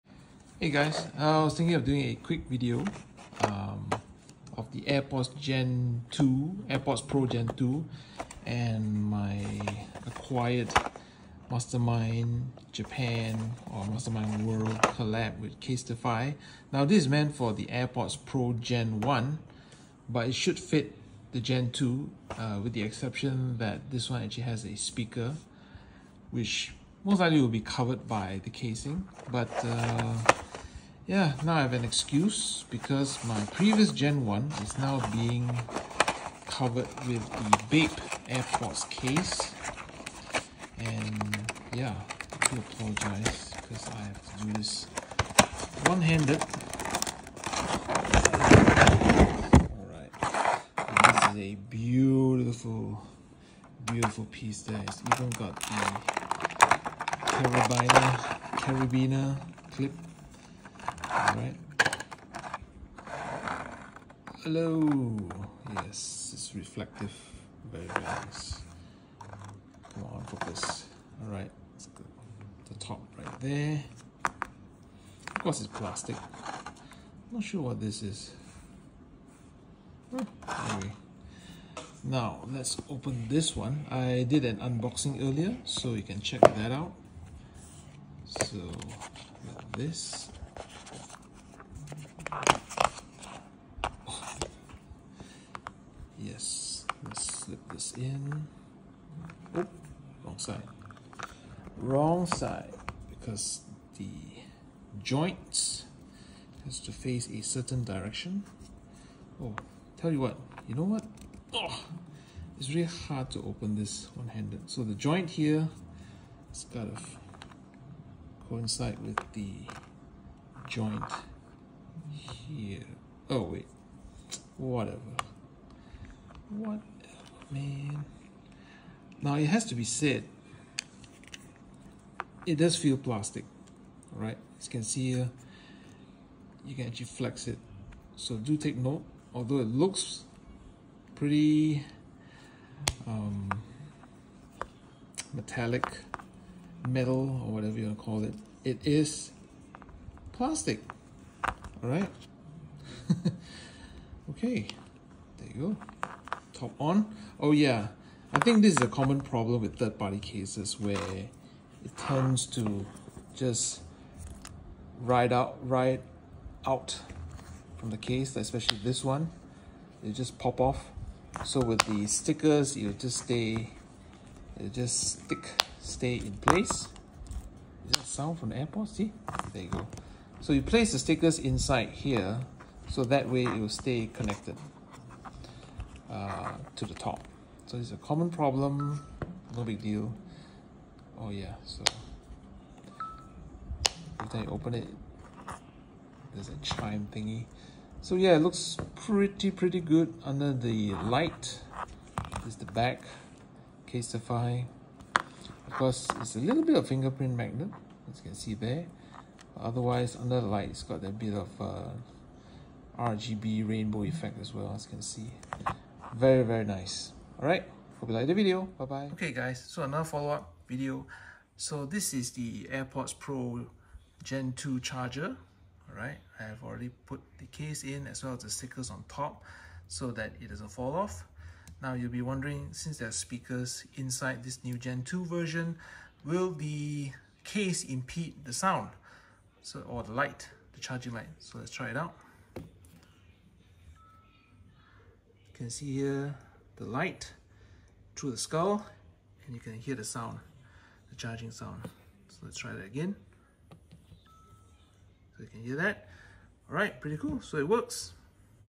Mastermind World x Casetify Airpods Pro Case Review & Gen2 fit!
The MMJ x Casetify Airpods Pro Case should fit functionally with the new Airpods Pro Gen 2  right? Let's go!